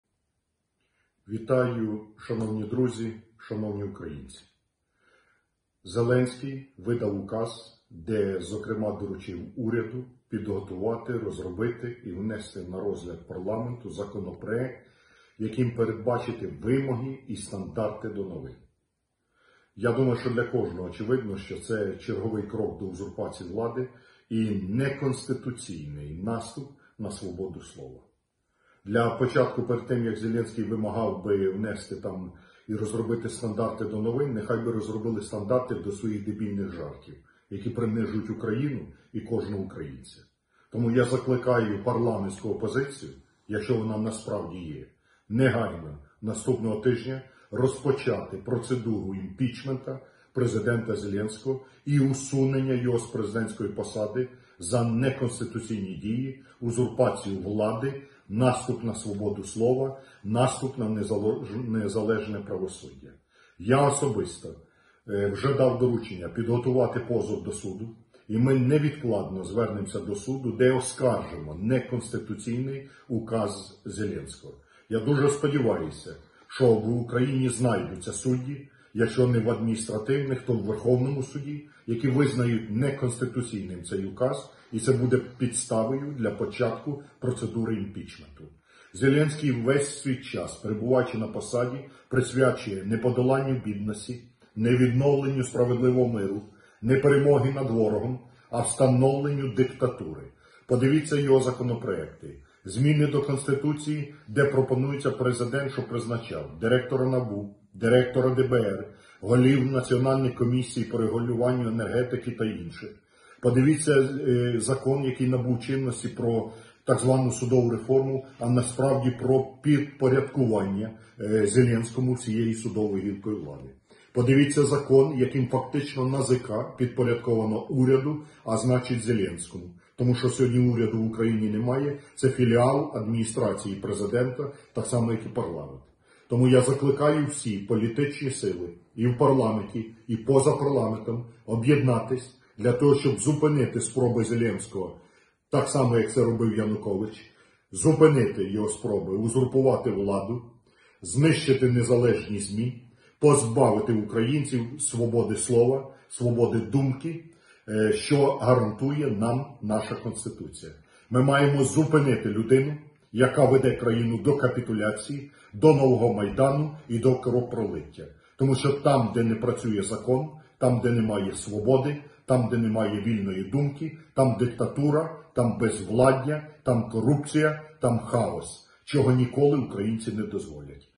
Ляшко вимагає ініціювати процедуру імпічменту Зеленському
Олег Ляшко записав відеозвернення до українців, в якому розкритикував доручення Володимира Зеленського Кабміну розробити та внести на розгляд Верховної Ради закон про вимоги та стандарти до новин. Політик вважає таку ініціативу неконституційним наступом на свободу слова та черговим кроком до узурпації влади. Лідер РПЛ вимагає від парламентської опозиції негайно ініціювати процедуру імпічменту Президента за його дії.
«Перед тим, як Зеленський вимагав би розробити стандарти до новин, нехай розробив би стандарти до своїх дебільних жартів, які принижують Україну і кожного українця. Тому я закликаю парламентську опозицію, якщо вона є, негайно, наступного тижня, розпочати процедуру імпічменту Президента Зеленського і усунення його з президентської посади за неконституційні дії, узурпацію влади, наступ на свободу слова і наступ на незалежне правосуддя», - звернувся до парламентарів Ляшко.
Політик повідомив, що планує звернутися до Верховного суду з позовом про визнання неконституційним законопроект Володимира Зеленського.
«Я особисто вже дав доручення підготувати позов і ми невідкладно звернемося до суду, де оскаржимо неконституційний указ Зеленського. Я дуже сподіваюся, що в Україні знайдуться судді, якщо не в адміністративних, то у Верховному суді, які визнають неконституційним цей указ. І це буде підставою для початку процедури імпічменту Президента», - вважає політик.
Олег Ляшко підкреслив, що перебуваючи на посаді Президента, Володимир Зеленський весь цей час присвячує не подоланню бідності, відновленню миру та поверненню територій, а встановленню диктатури. Свідченням цього вважає його законодавчі ініціативи, зокрема, закон про судову реформу.
«Це закон, яким фактично НАЗК підпорядковано уряду, а значить – Зеленському, тому що сьогодні уряду в Україні фактично немає – це філіал ОП, так само, як і парламент. Тому я закликаю всі політичні сили і в парламенті, і поза ним об’єднатися для того, щоб зупинити спробу Зеленського узурпувати владу, знищити незалежні ЗМІ, позбавити українців свободи слова, свободи думки, що гарантує нам наша Конституція. Ми маємо зупинити людину, яка веде країну до капітуляції, нового Майдану і кровопролиття, тому що там, де не працює закон, там, де немає свободи і вільної думки - там диктатура, безвладдя, хаос, чого ніколи українці не дозволять!», - підсумував Ляшко.